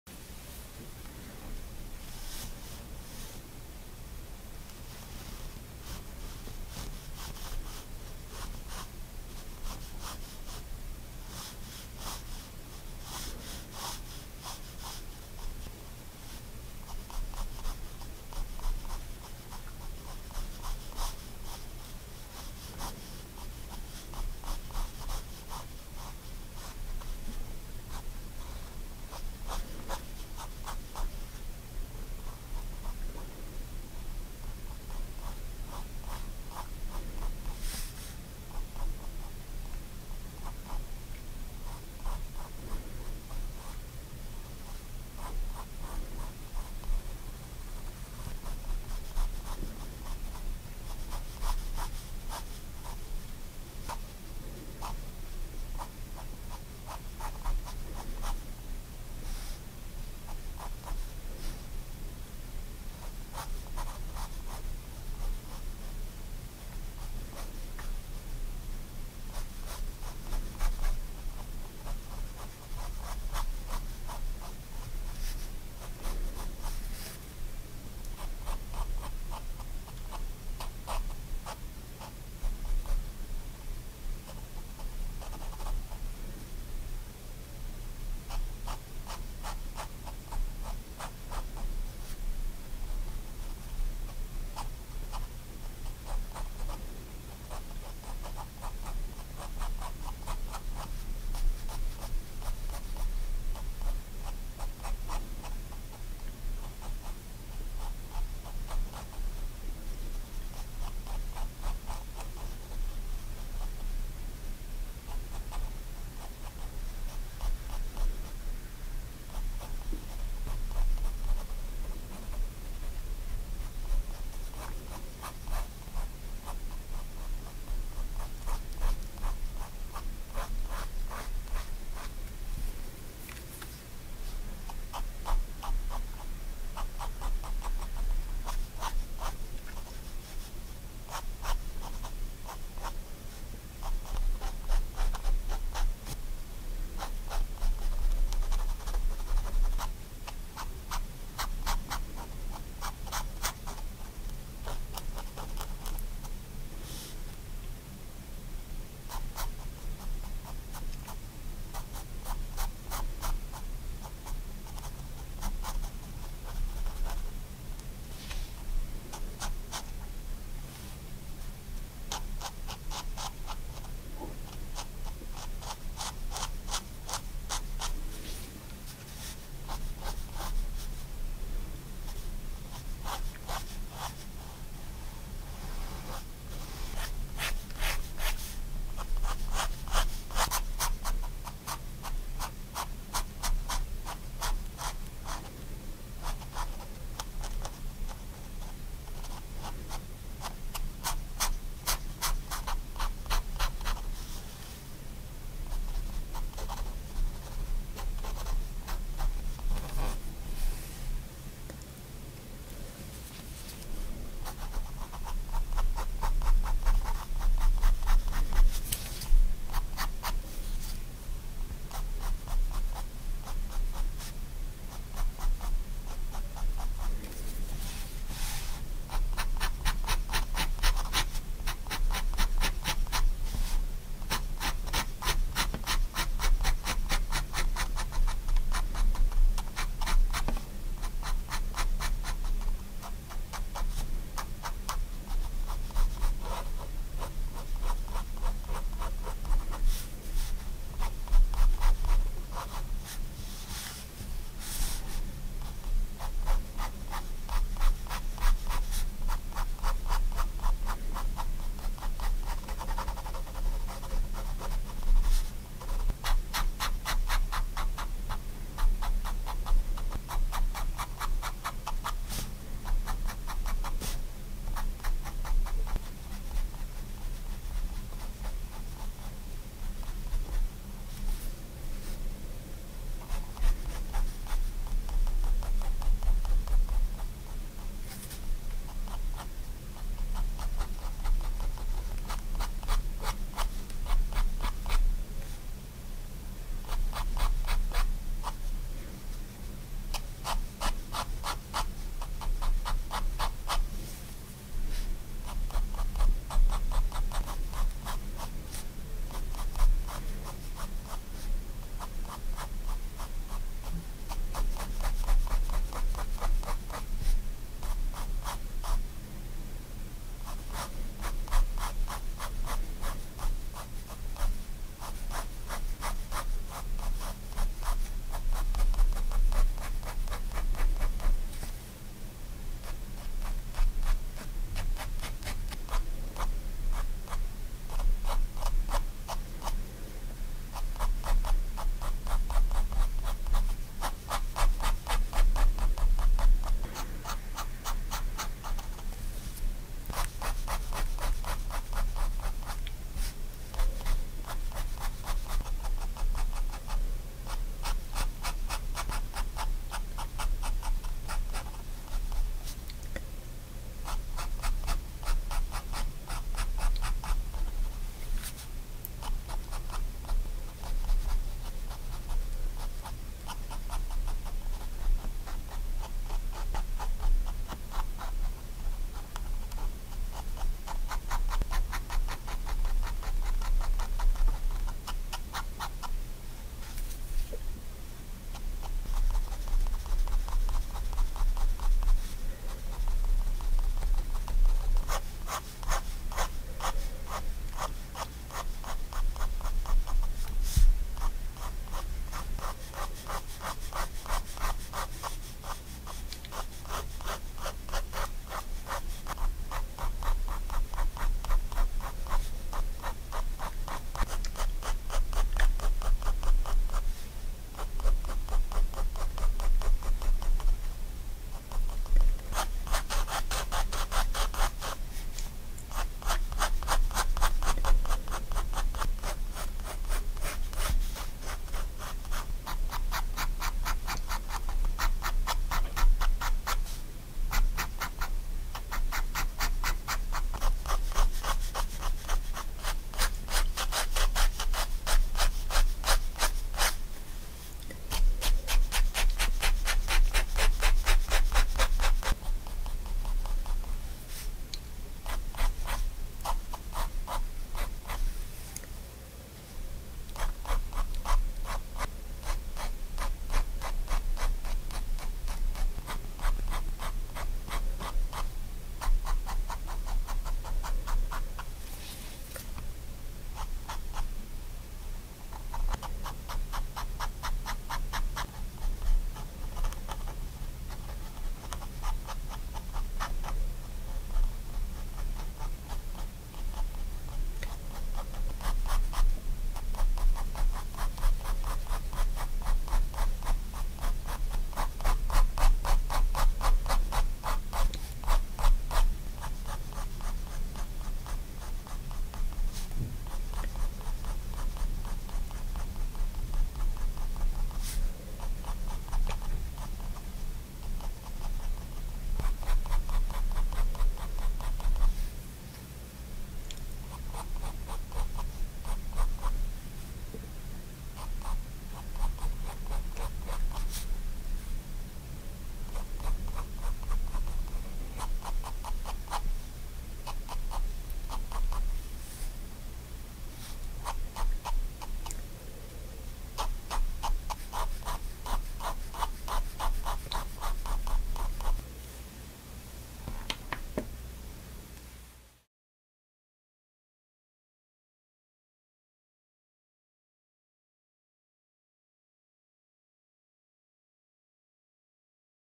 The Creation of Adam hands - pen drawing sounds ASMR - sketch step by step
STAEDTLER pigment liner 0.1mm